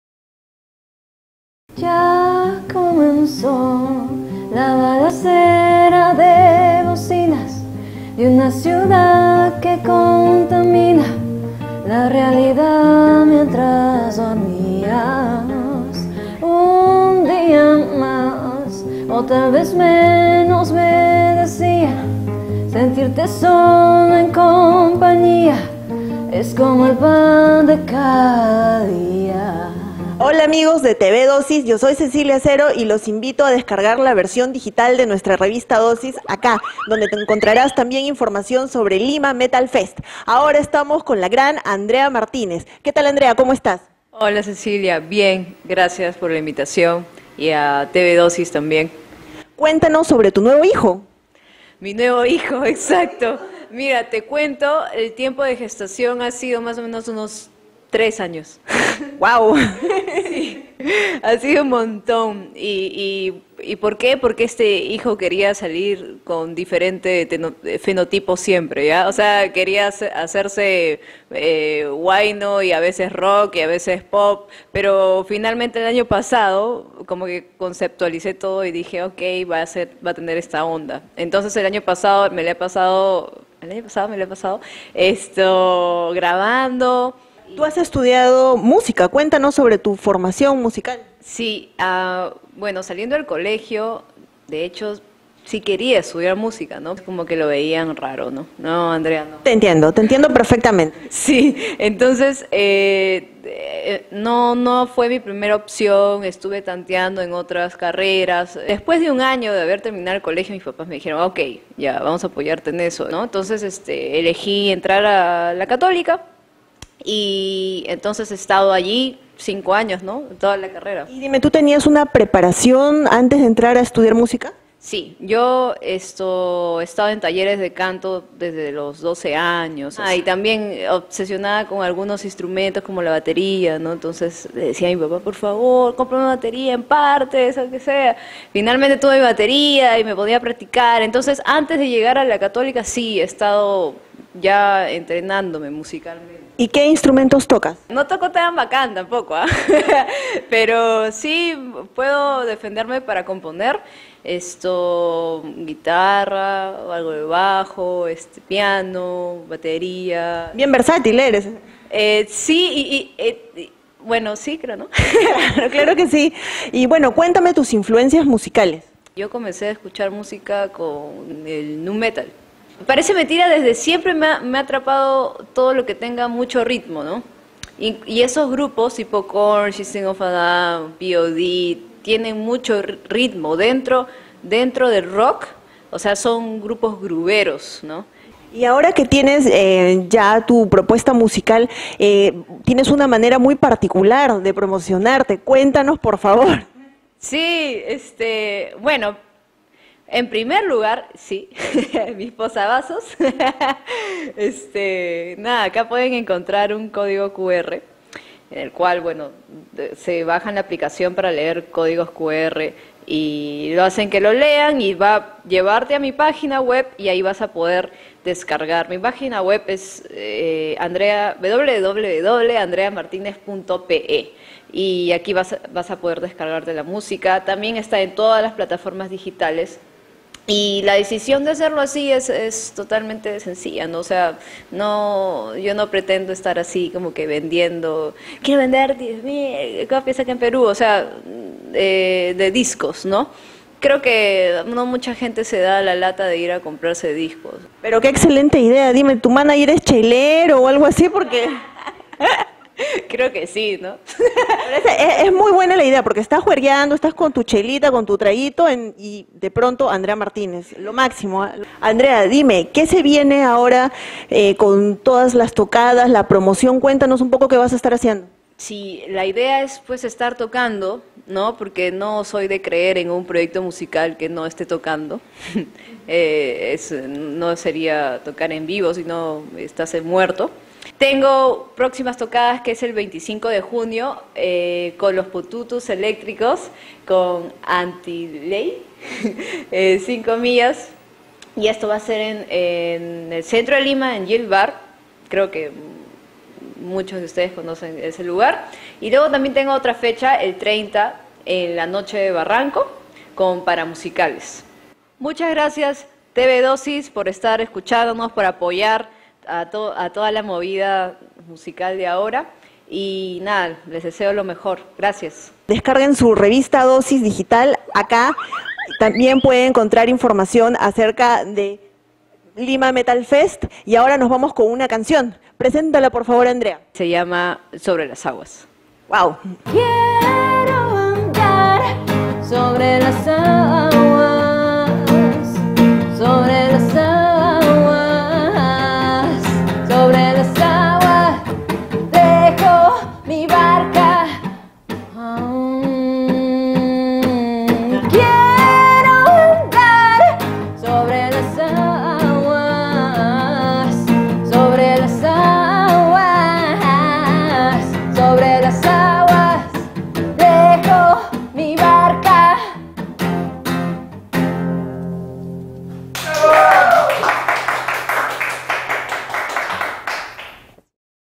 TVDOSIS: ENTREVISTA A ANDREA MARTÍNEZ (CANTAUTORA)
Amena conversa con la cantautora peruana Andrea Martínez, que acaba de lanzar un disco mediante una novedosa propuesta digital. Entérate aquí en TVDOSIS.